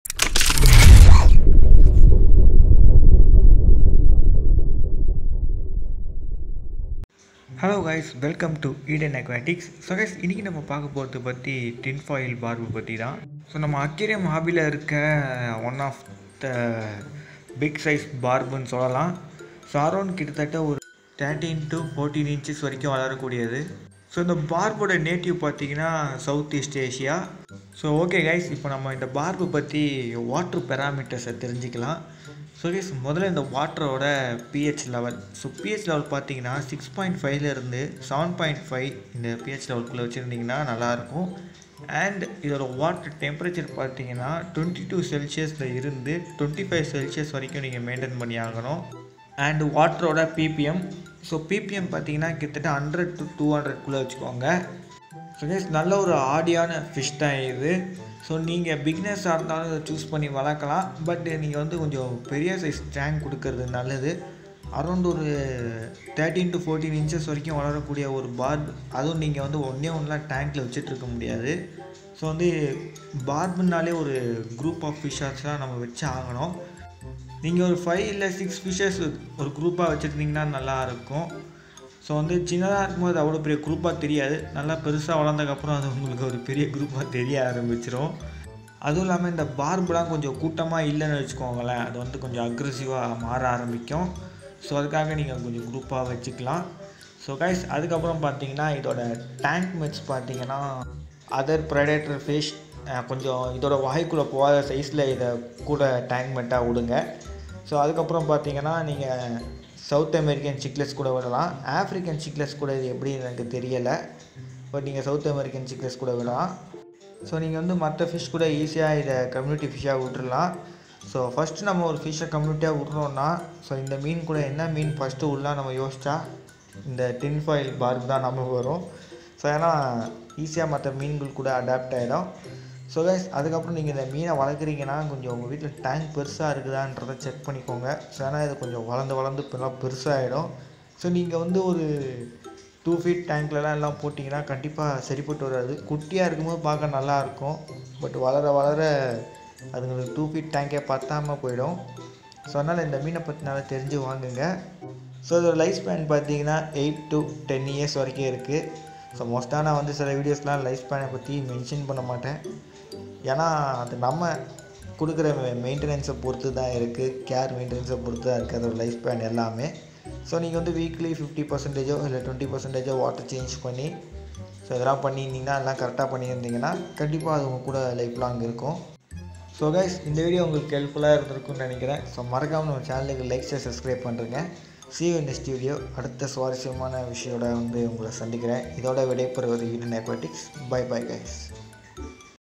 Tinfoil Barb Maintenance || Aquarium fish || Tamil
The tinfoil barb is a tropical Southeast Asian freshwater fish of the family Cyprinidae. This species was originally described as Barbus schwanenfeldii by Pieter Bleeker in 1853, and has also been placed in the genera Barbodes and Puntius. The specific epithet is frequently misspelled schwanefeldii.